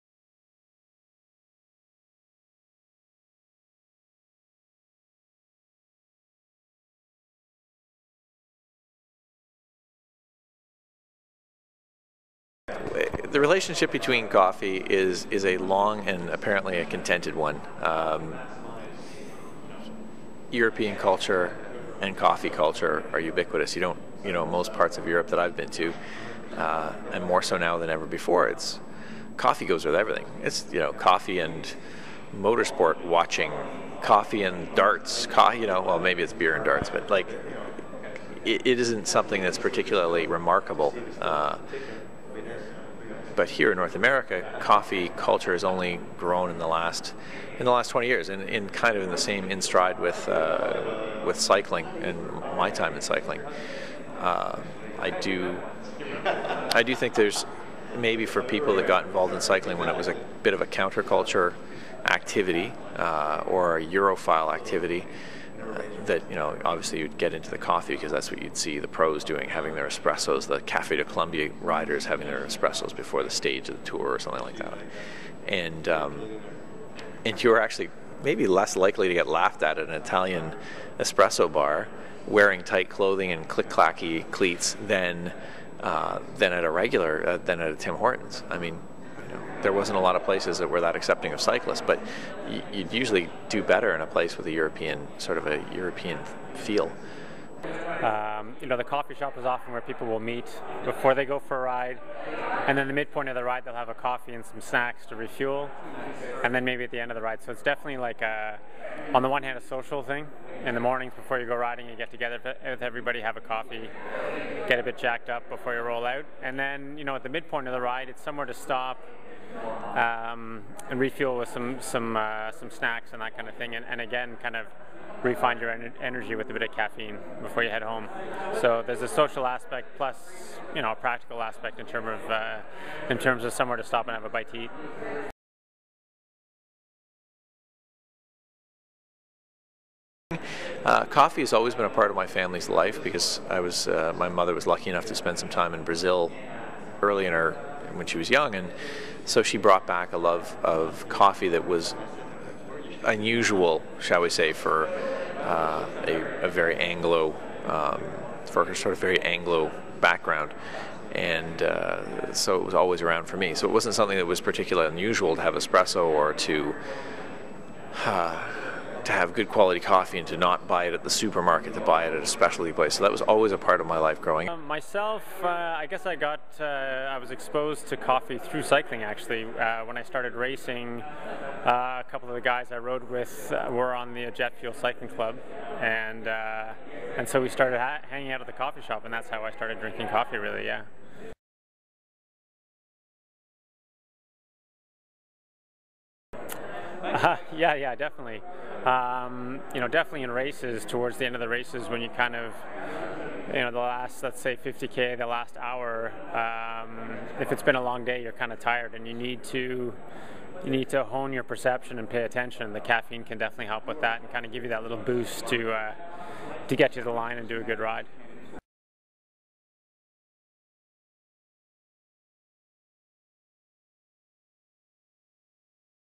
On Coffee and Cycling
Domestique Cafe owner Krys Hines and former pro rider Andrew Randell talk java and bikes